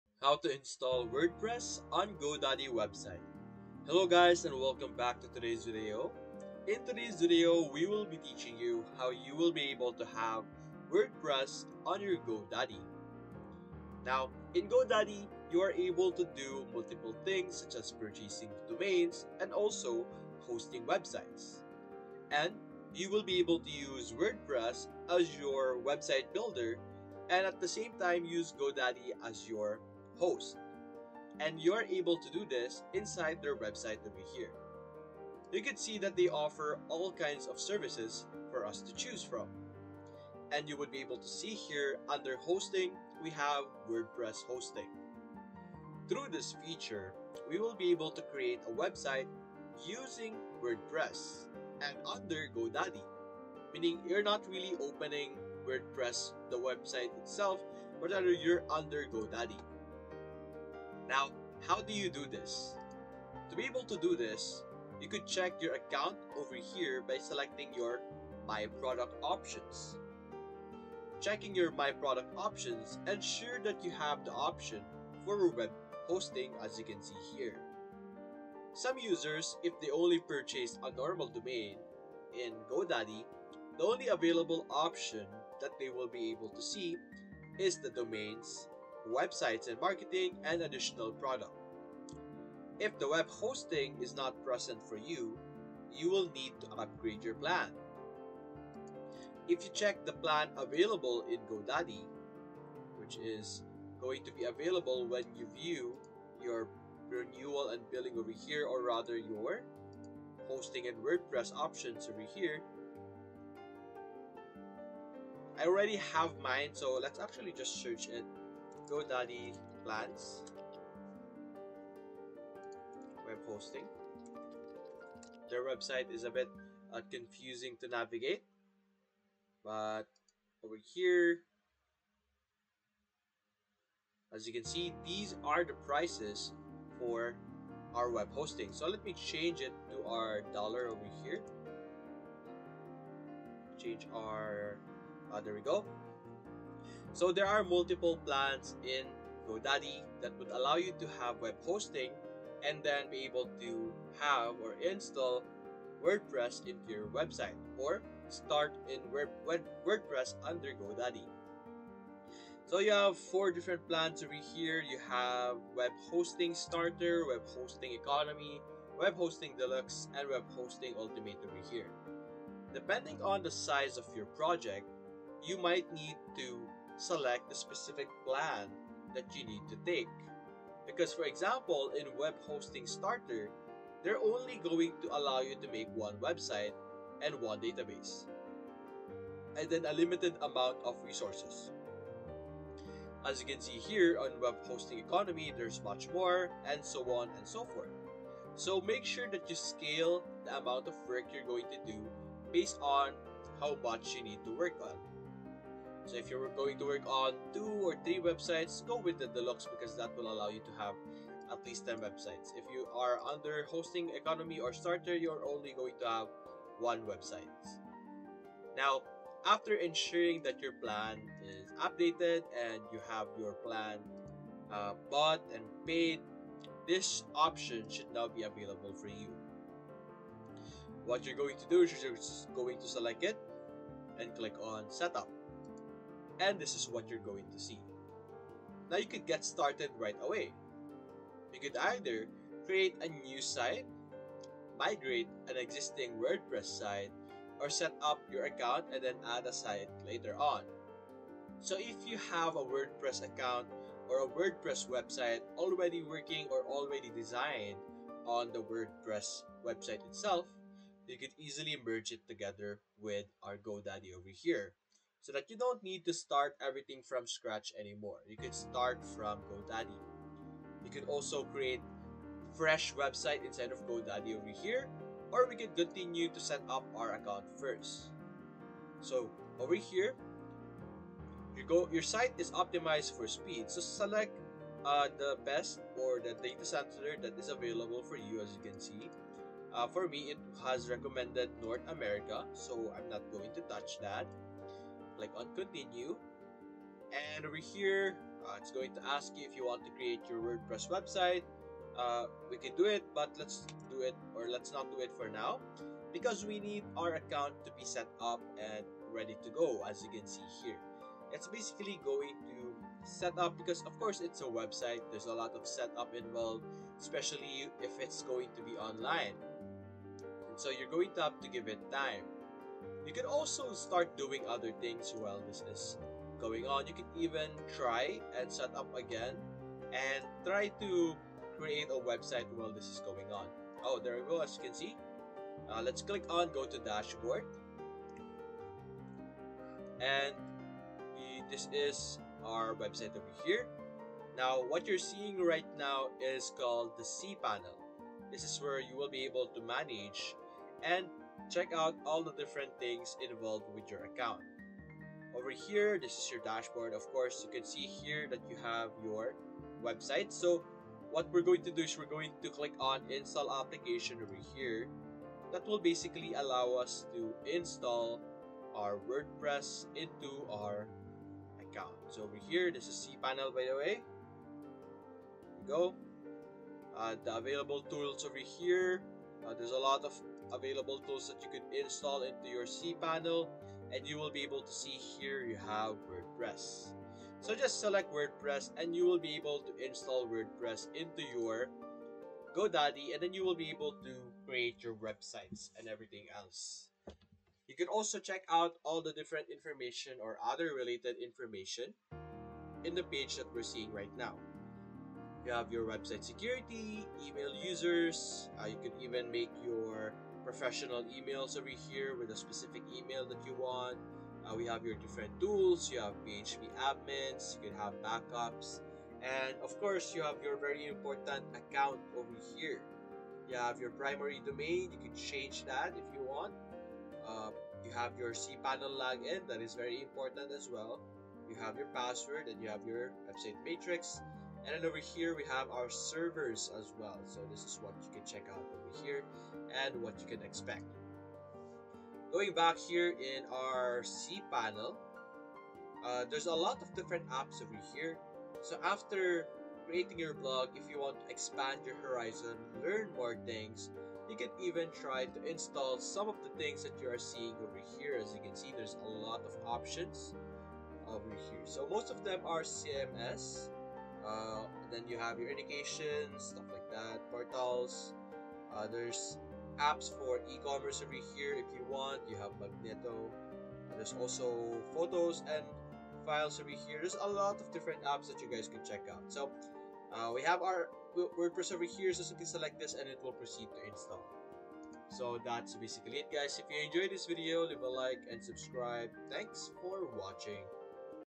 How to Install Wordpress on Godaddy Website (Full Guide)
Learn how to install WordPress on your GoDaddy website with this full guide. Follow along step by step to set up your site easily and efficiently.